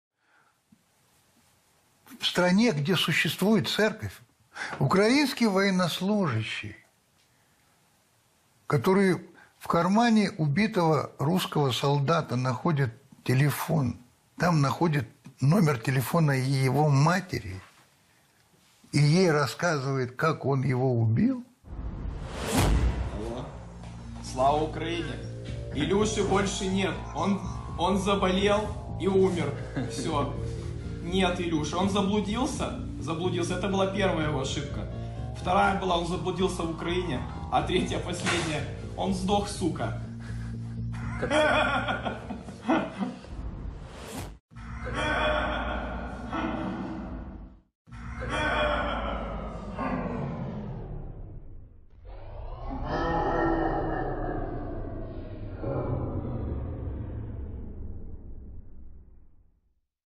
Ukrajinský vojak hovorí s výsmechom matke ruského vojaka, že jej syn zdochol | 2022
V krajine, kde existuje cirkev, Ukrajinský vojak, ktorý vo vrecku zabitého ruského vojaka našiel mobilný telefón, aj číslo jeho matky, s ktorou sa cezeň spojil, jej hovorí, ako ho zabil?: ,,Sláva Ukrajine, Iľjušu viac niet. Ochorel a zomrel. To je všetko. Už niet Iľjušu. Zablúdil, to bola jeho prvá chyba. Druhá chyba bola, že zablúdil na Ukrajinu, a tretia posledná, že zdochol suka. Hahahahaha!”